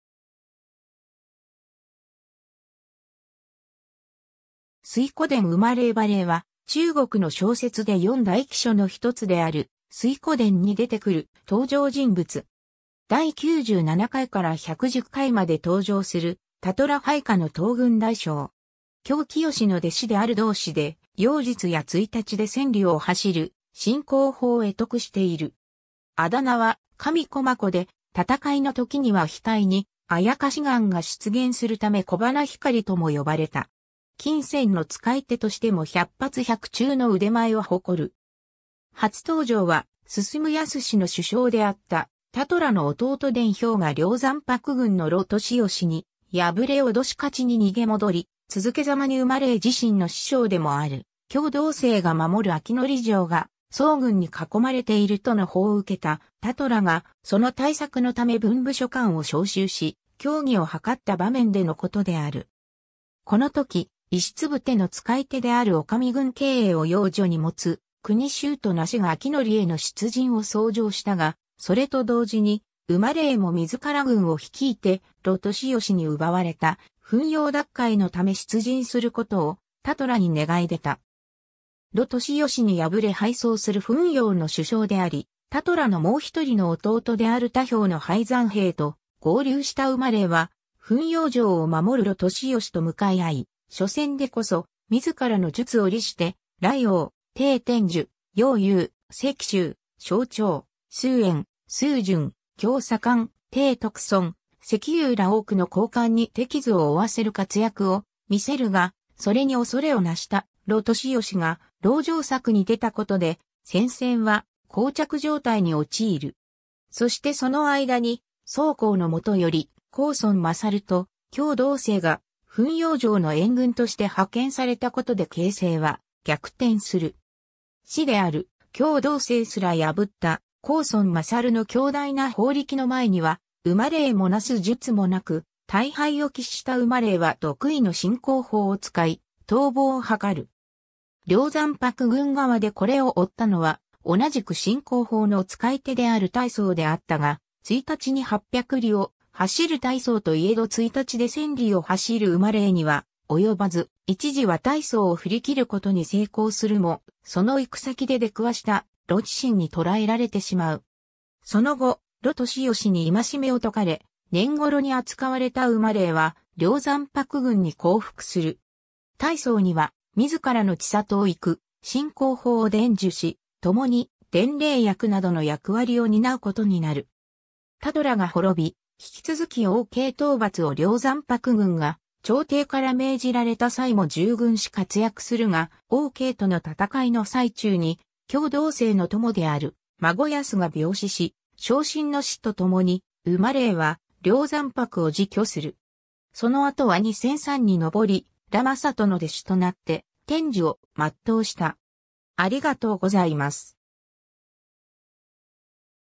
馬霊
水滸伝 」 馬霊 馬 霊（ば れい）は、中国の小説で四大奇書の一つである『水滸伝』に出てくる登場人物。
 第97回から110回まで登場する田虎配下の統軍大将。
喬冽の弟子である道士で、妖術や一日で千里を走る神行法を会得している。
渾名は神駒子（しんくし）で、戦いの時には額に妖眼が出現するため小華光（しょうかこう）とも呼ばれた。
金磚（きんせん）の使い手としても百発百中の腕前を誇る。
 初登場は、晋寧の守将であった田虎の弟田彪が梁山泊軍の盧俊義に敗れ威勝に逃げ戻り、続けざまに馬霊自身の師匠でもある喬道清が守る昭徳城が宋軍に囲まれているとの報を受けた田虎が、その対策のため文武諸官を招集し協議を図った場面でのことである。
この時、石礫の使い手である女将軍瓊英を養女に持つ国舅鄔梨が昭徳への出陣を奏上したが、それと同時に馬霊も自ら軍を率いて盧俊義に奪われた汾陽奪回のため出陣することを、田虎に願い出た。
 盧俊義に敗れ敗走する汾陽の守将であり、田虎のもう一人の弟である田豹の敗残兵と合流した馬霊は、汾陽城を守る盧俊義と向かい合い、緒戦でこそ自らの術を利して、雷横、鄭天寿、楊雄、石秀、焦挺、鄒淵、鄒潤、龔旺、丁得孫、石勇ら多くの好漢に手傷を負わせる活躍を見せるが、それに恐れをなした盧俊義が籠城策に出たことで、戦線は膠着状態に陥る。
そしてその間に、宋江の元より公孫勝と喬道清が汾陽城の援軍として派遣されたことで形成は逆転する。
師である喬道清すら破った公孫勝の強大な法力の前には馬霊もなす術もなく、大敗を喫した馬霊は得意の神行法を使い逃亡を図る。
梁山泊軍側でこれを追ったのは、同じく神行法の使い手である戴宗であったが、一日に八百里を走る戴宗といえど一日で千里を走る馬霊には及ばず、一時は戴宗を振り切ることに成功するも、その行く先で出くわした魯智深に捕えられてしまう。
その後、盧俊義に縛めを解かれ、懇ろに扱われた馬霊は梁山泊軍に降伏する。
戴宗には自らの千里を行く神行法を伝授し、共に伝令役などの役割を担うことになる。
 田虎が滅び、引き続き王慶討伐を梁山泊軍が朝廷から命じられた際も従軍し活躍するが、王慶との戦いの最中に喬道清の友である孫安が病死し、傷心の師と共に馬霊は梁山泊を辞去する。
その後は二仙山に上り、羅真人の弟子となって天寿を全うした。